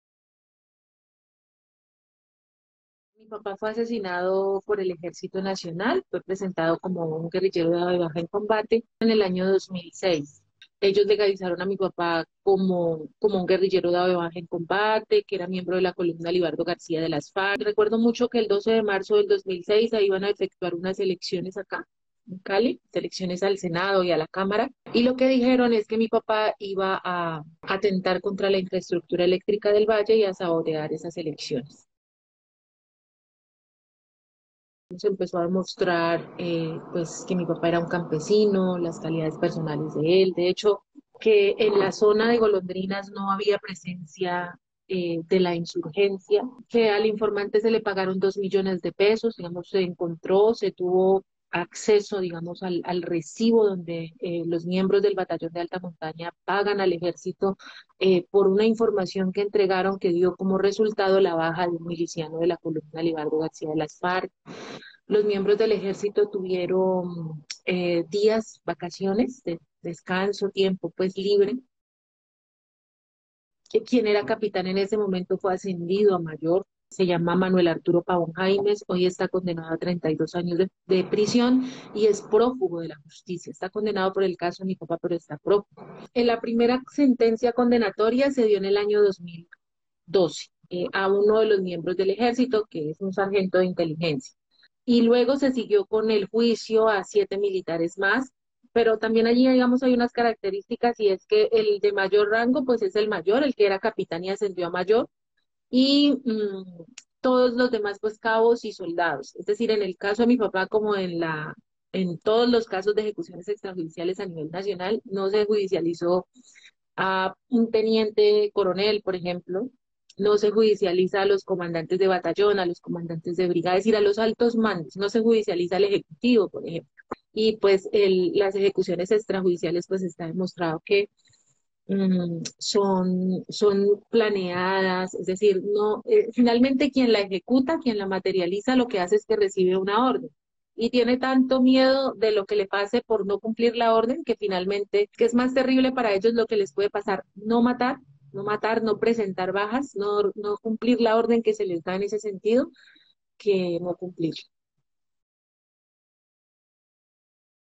EjecucionesExtrajudiciales El caso de José Orlando Giraldo
La Jurisdicción Especial para la Paz -JEP- reveló que la cifra de ejecuciones extrajudiciales es de 6.402 y no de 2.248 como lo había reportado la Fiscalía de Colombia.

Además, dice la JEP que de "2002 a 2008 se registró el 78% del total de la victimización histórica" es decir, durante el gobierno de Álvaro Uribe Vélez.

A propósito de ello, les compartimos un fragmento de la charla  que sostuvimos en diciembre con Martha Giraldo, representante del Movimiento Nacional de Víctimas de Estado, Capitulo Valle del Cauca. y quien es hija de un campesino, víctima de dicho crimen.